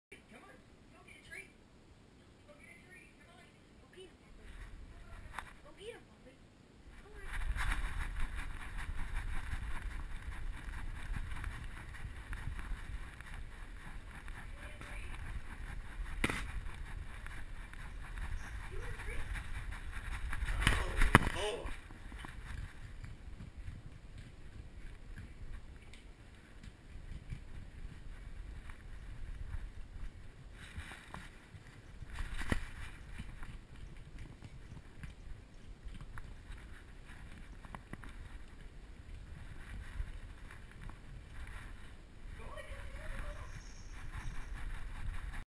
Puppycam: Puppy Gets a Treat
I strapped my GoPro Hero2 to Puppy and called her to the kitchen for a treat.